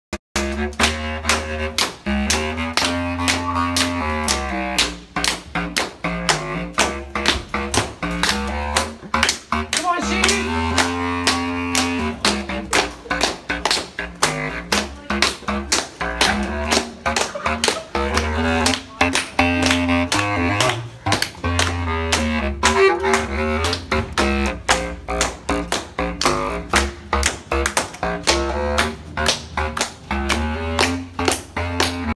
Contrabass, clarinet sound
Sound of a contrabass clarinet that is a huge instrument